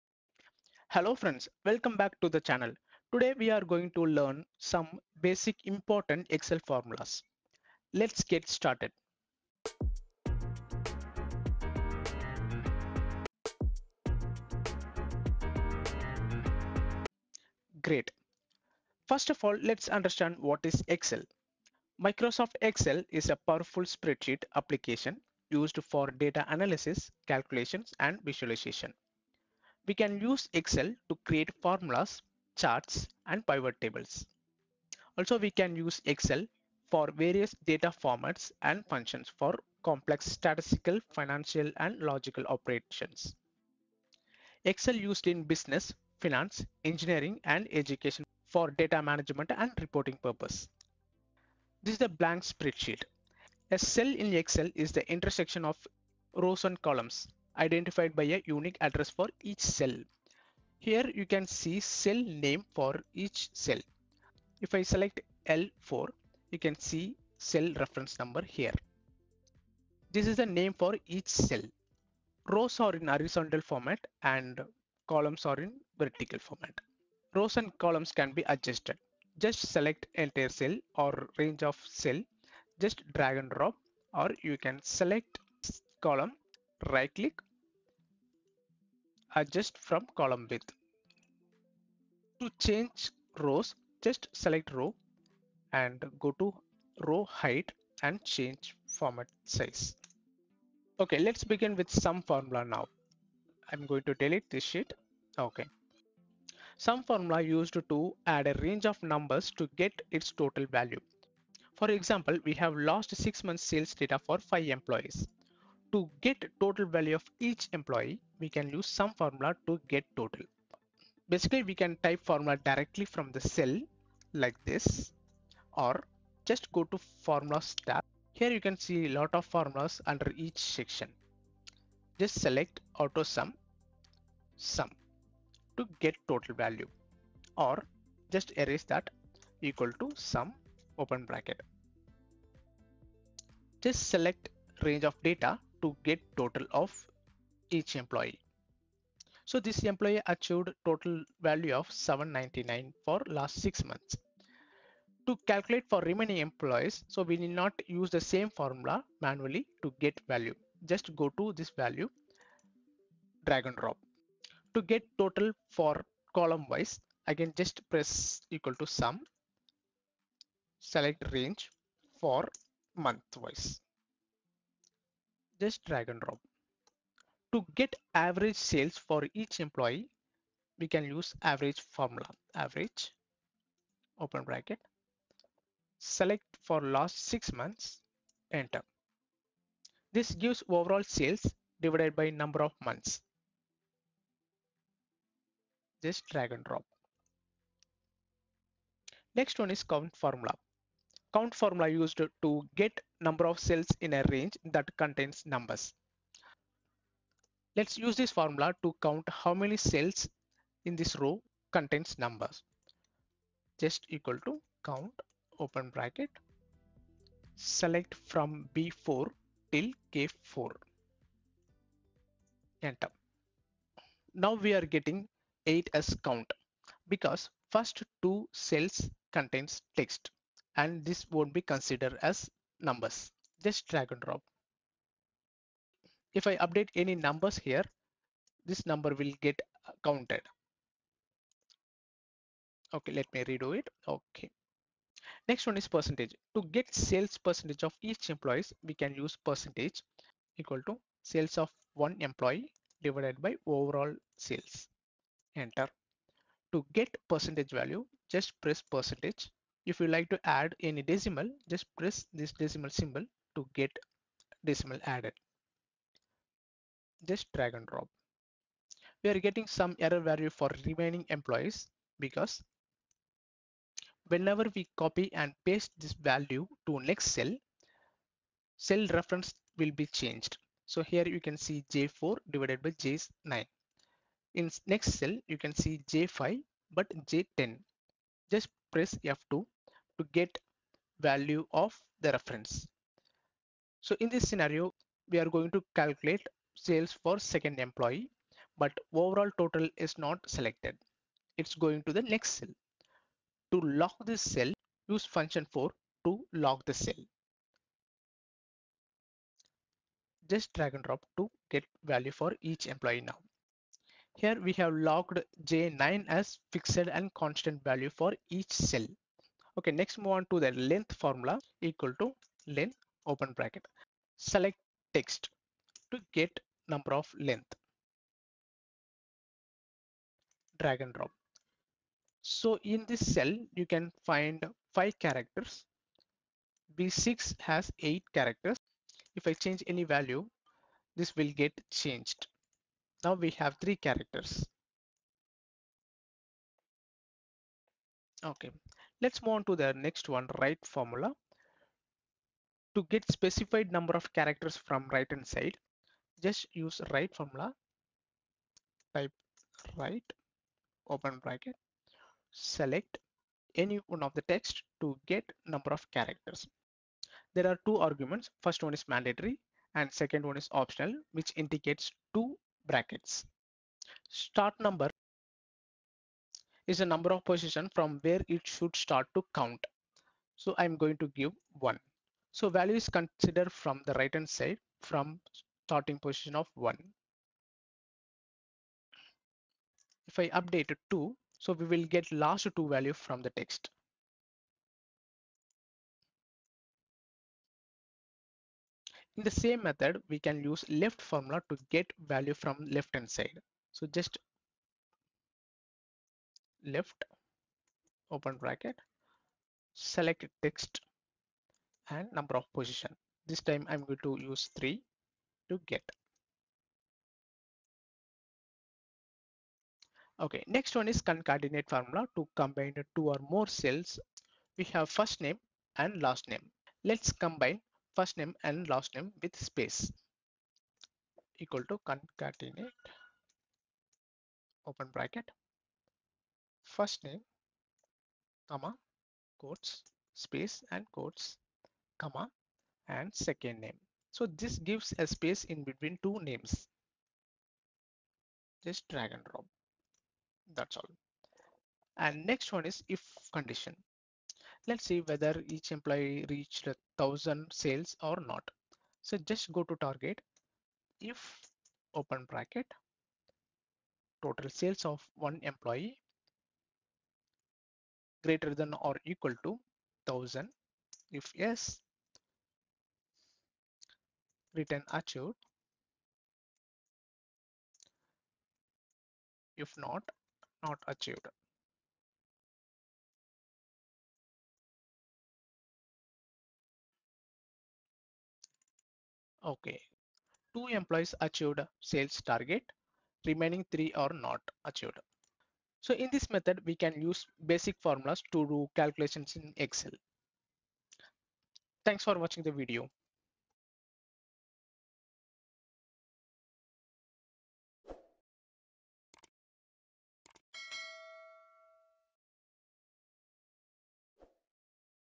Important Basic Excel Formulas for Beginners
In this video, we will be learning following formulas.

SUM - The `SUM` formula in Excel adds together all the numbers in a specified range. You use it by typing `=SUM(range).

AVERAGE - The `AVERAGE` formula in Excel calculates the mean of the numbers in a specified range, used by typing `=AVERAGE(range)`.

COUNT - The `COUNT` formula in Excel returns the number of cells containing numerical values in a specified range, using `=COUNT(range)`.

PERCENTAGE - To calculate a percentage in Excel, you can divide one value by another and multiply by 100, typically using a formula like `=(part/total)*100`.

LEN - The `LEN` formula in Excel returns the number of characters in a text string, including spaces, using `=LEN(text)`.

RIGHT & LEFT - The `RIGHT` formula in Excel extracts a specified number of characters from the end of a text string, using `=RIGHT(text, num_chars)`, while the `LEFT` formula extracts a specified number of characters from the beginning of a text string, using `=LEFT(text, num_chars)`.

CONCATENATE - The `CONCATENATE` formula in Excel joins two or more text strings into one, using `=CONCATENATE(text1, text2, ...)`.

IF FORMULA -

Recent Useful Videos to watch !!!